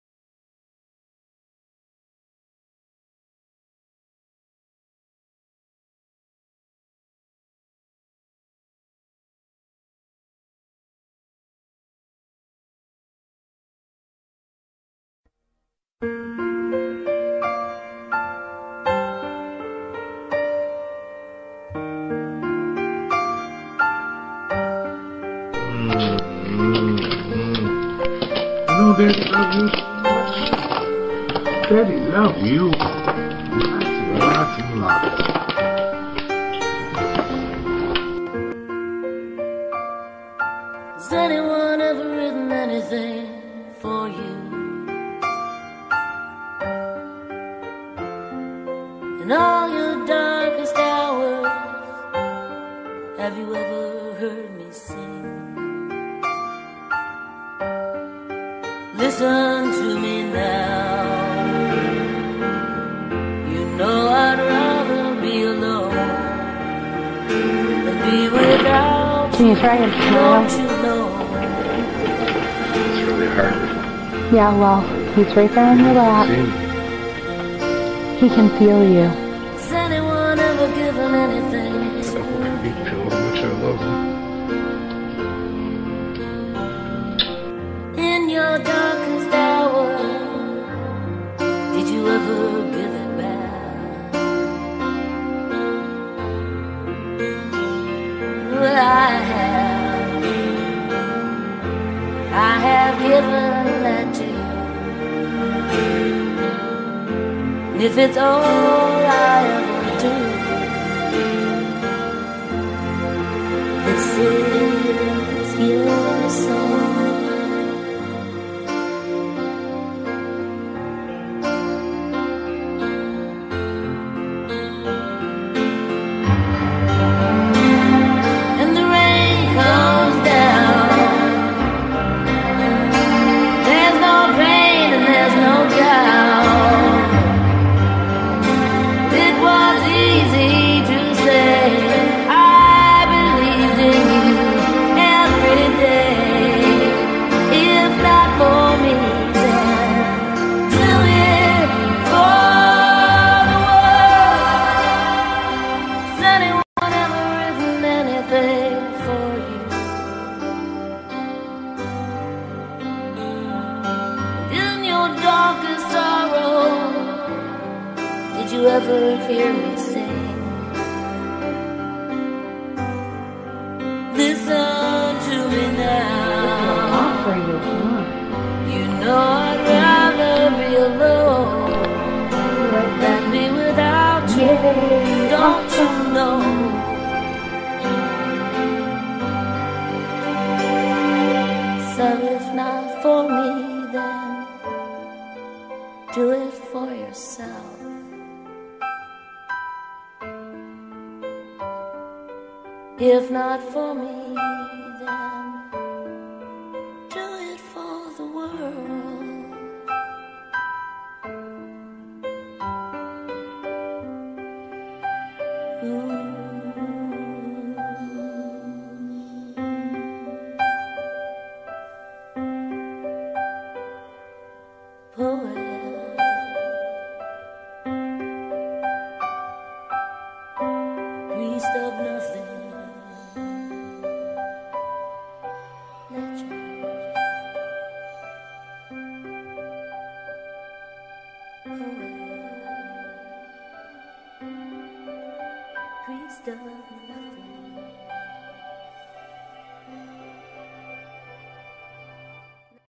Dedicated to RJ Tough Guy Kaufman and all the Children who have Battled Childhood cancer
The Song is by Stevie Nicks from her latest works